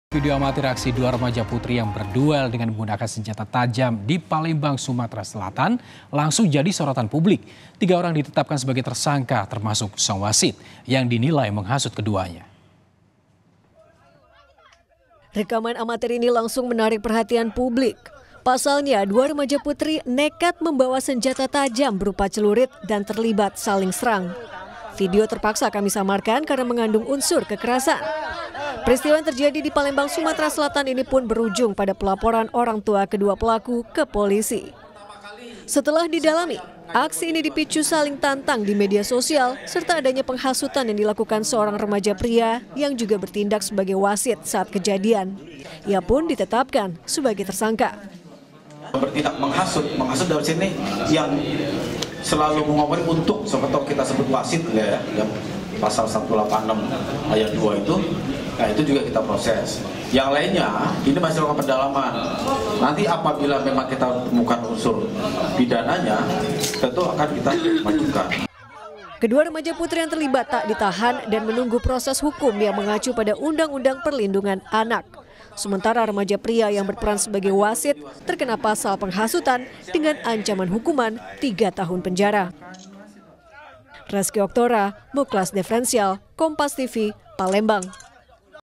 Miris! Remaja Putri di Palembang Duel Pakai Senjata Tajam, 3 Orang Jadi Tersangka!
PALEMBANG, KOMPAS.TV - Video amatir aksi dua remaja putri yang berduel dengan menggunakan senjata tajam di Palembang, Sumatera Selatan langsung jadi sorotan publik. Tiga orang ditetapkan sebagai tersangka termasuk sang wasit yang dinilai menghasut keduanya.

Rekaman amatir ini langsung menarik perhatian publik. Pasalnya dua remaja putri nekat membawa senjata tajam berupa celurit dan terlibat saling serang.  Video terpaksa kami samarkan karena mengandung unsur kekerasan.

Peristiwa yang terjadi di Palembang Sumatera Selatan ini pun berujung pada pelaporan orangtua kedua pelaku ke polisi.